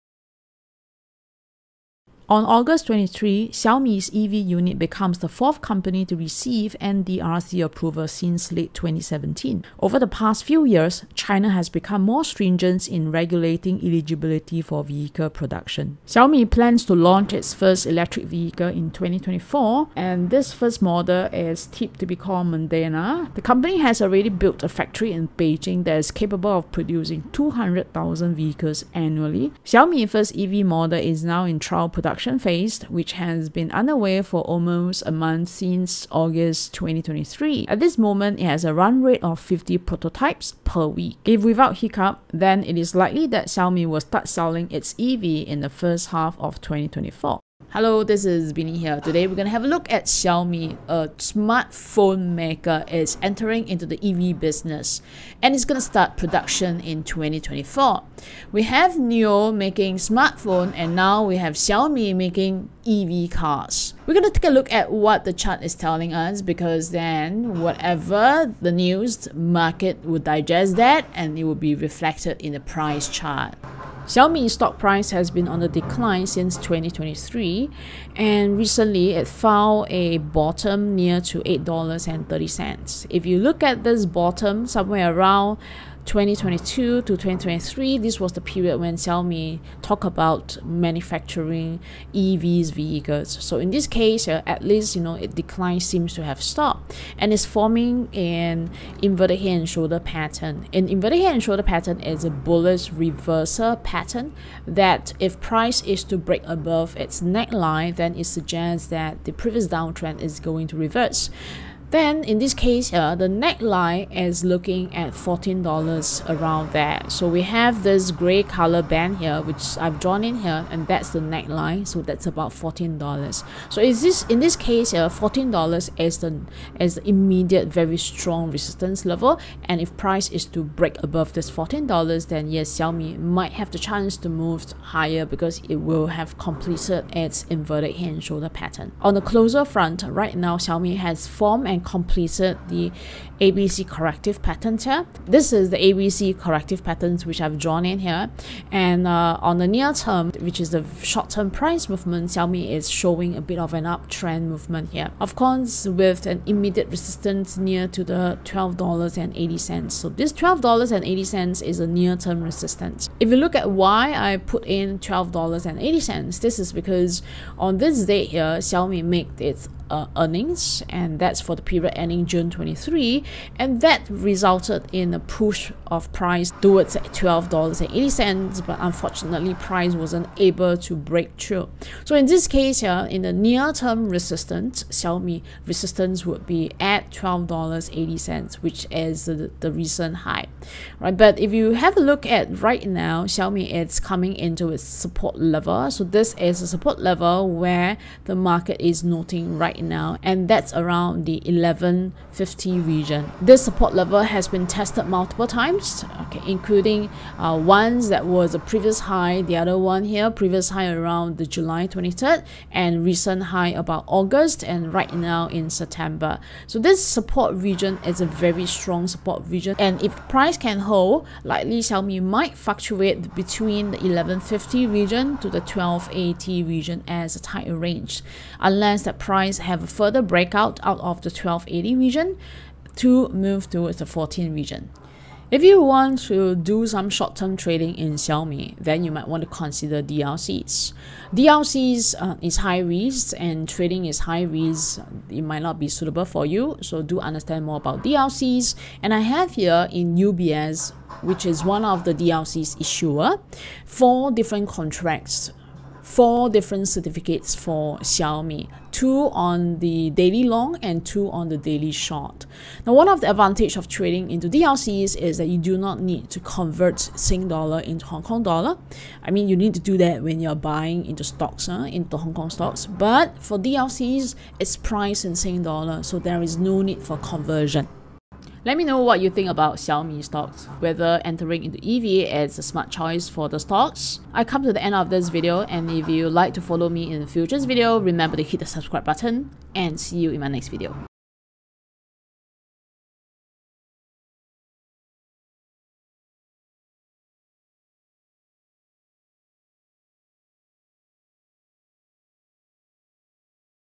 Will Xiaomi's $10 Billion Electric Vehicle Investment Boost The Stock?
We take a close look at Xiaomi's venture into the electric vehicle market and its impact on the company's stock price. We discuss whether the market likes Xiaomi's entry into the EV business and what it means for the future of the EV market. Xiaomi has officially registered its electric vehicle business and pledged an investment of $10 billion over the next decade.

In this video, Binni revealed a very important chart pattern on Xiaomi and explained its use for short to mid-term trading. If you are interested, you might want to explore Daily Leverage Certificates for short-term trading using the key levels mentioned.

00:00 Introduction
01:13 XIAOMI Chart
03:55 UBS DLC
04:38 Ending remarks